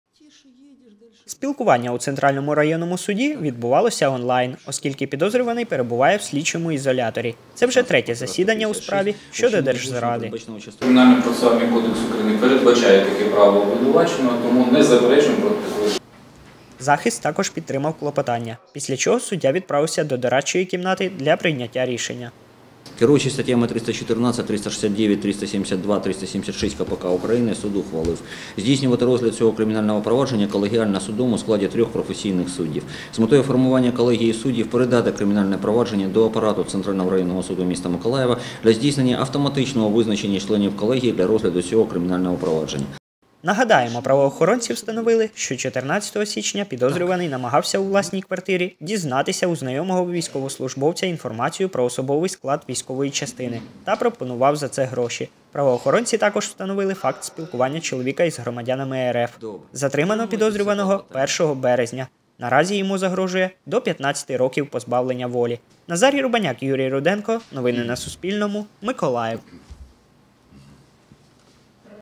Миколаївський суд постановив задовольнити клопотання підозрюваного у державній зраді
Миколаївський Центральний суд постановив задовольнити клопотання обвинуваченого в держзраді щодо участі у процесі колегії у складі трьох суддів. Кримінальне провадження передано до апарату Центрального районного суду міста Миколаєва для здійснення автоматичного визначення членів колегії.